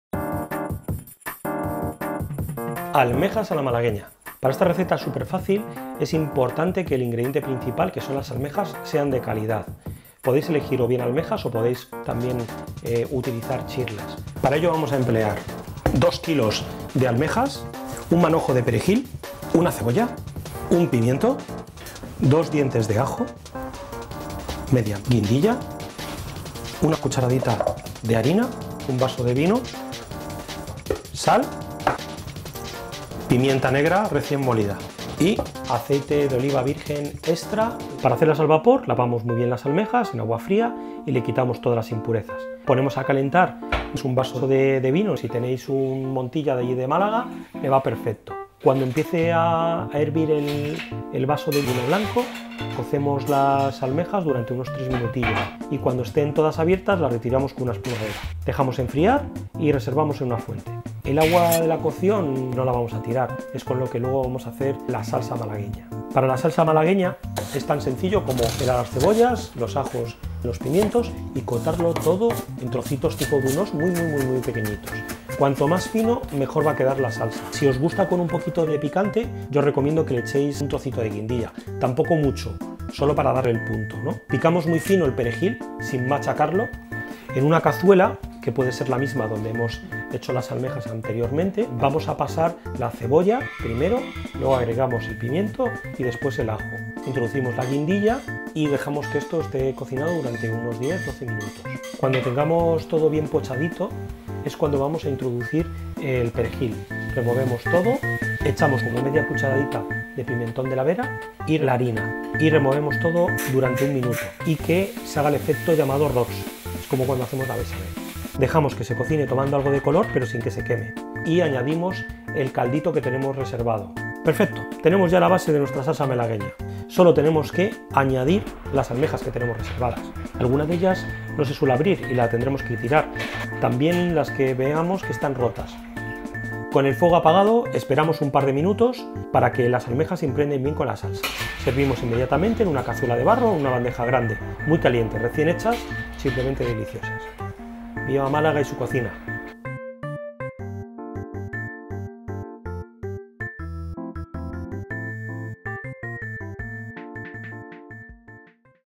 Receta sencilla de almejas a la malagueña.- 'De Rechupete'
Hoy en 'De Rechupete' el maravilloso chef Alfonso Díaz, nos trae una fantástica receta de 'almejas a la malagüeña' que sorprenderá a tu paladar y que podrás realizar, o bien con almejas, o bien con chirlas.¡No te lo puedes perder!

Ingredientes:

-2kg. De almejas
-1 manojo de perejil
-1 cebolla
-1 pimiento
-2 dientes de ajo
-1/2 guindilla
-1 cucharadita de harina
-1 vaso de vino
-Sal
-Pimienta negra

Elaboración:

1.- Lavamos las almejas bien en agua fría y les quitamos las impurezas.

2.- Ponemos a calentar un vaso de vino blanco. Cuando empiece a hervir, cocemos las almejas durante unos 3 minutillos y, cuando estén todas abiertas, las sacamos con una espumadera y las reservamos en una fuente.

3.-Pelamos las cebollas, los ajos y los pimientos en trozos muy finos, lo máximo posible. Si os gusta el picante, echamos un poquito de guindilla.

4.- En una cazuela pasamos la cebolla, luego el pimiento y después el ajo. Si hemos utilizado guindilla, también esta y dejamos que todo se cueza durante aproximadamente 10 minutos. Cuando esté todo bien 'pochadito' es cuando vamos a introducir el perejil picado. Removemos todo, echamos media cucharadita de pimentón de la vera y harina y removemos todo durante un minuto.

5.- Dejamos que se cocine adquiriendo algo de color pero sin tostarse y añadimos el vino blanco que hemos reservado previamente.

6.- Añadimos las almejas, con cuidado de retirar las que no se hayan abierto o las que estén rotas, echando también el jugo que habrán soltado.

7.- Esperamos unos minutos con el fuego apagado y servimos inmediatamente muy calientes, en una cazuela de barro o plato.